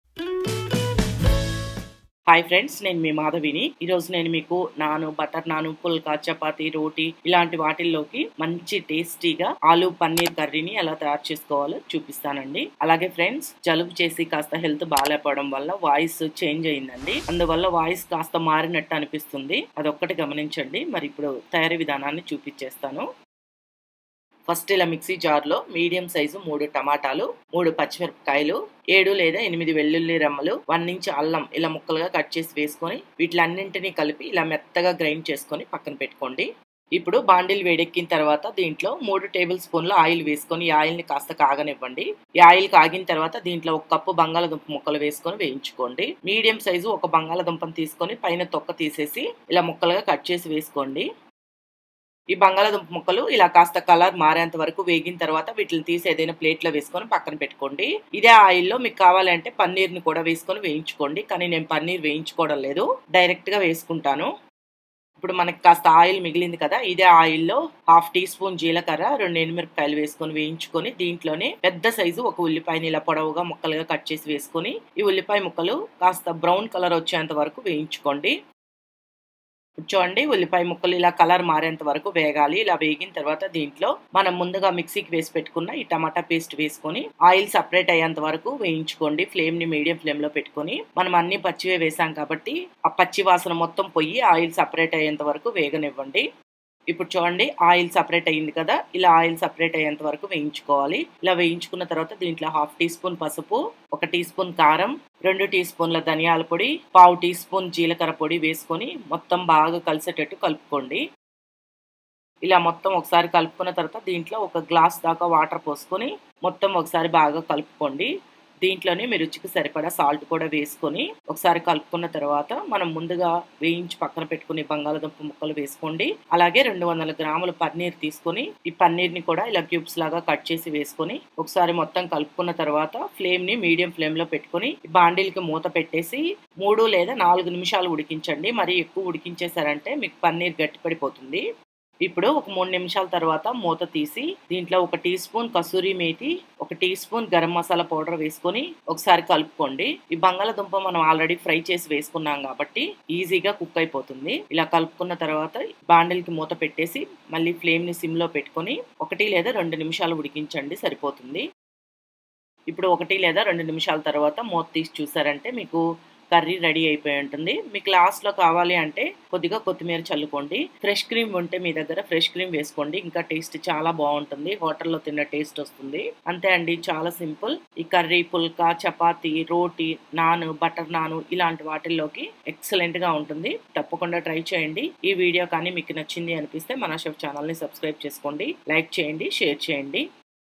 రైస్ రోటి పూరీలలోకి సూపర్ టేస్టీ ఆలూ పనీర్ కర్రీ Aloo Paneer Curry Telugu
Here is the Aloo Paneer Curry Recipe which can be suitable for Rice, Chapati, and Puri, do try it at home.